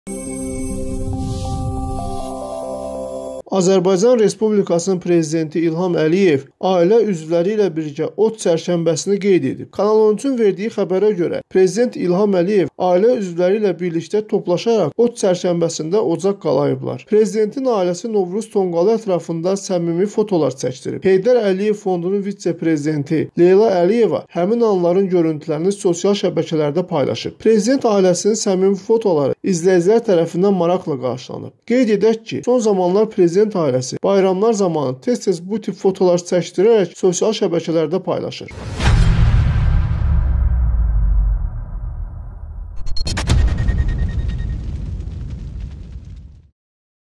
İlham Əliyev ailəsi ilə birgə Novruz tonaqlı ətrafında-FOTOlar
Azərbaycan Respublikasının Prezidenti İlham Əliyev ailə üzvləri  ilə birlikdə Od çərşənbəsini qeyd edib.Kanal13-ün verdiyi xəbərə görə,  Prezident İlham Əliyev  ailə üzvləri ilə birlikdə toplaşaraq od çərşənbəsində ocaq qalayıblar. Prezidentin  ailəsi novruz tonqalı ətrafında səmimi fotolar çəkdirib. Heydər Əliyev Fondunun vitse-prezidenti Leyla Əliyeva həmin anların görüntülərini sosial şəbəkədə paylaşıb.Prezident ailəsinin səmimi fotoları izləyicilər tərəfindən maraqla qarşılanıb. Qeyd edək ki, son zamanlar prezident ailəsi bayramlar zamanı tez tez bu tip fotolar çəkdirərək sosial şəbəkələrdə paylaşır.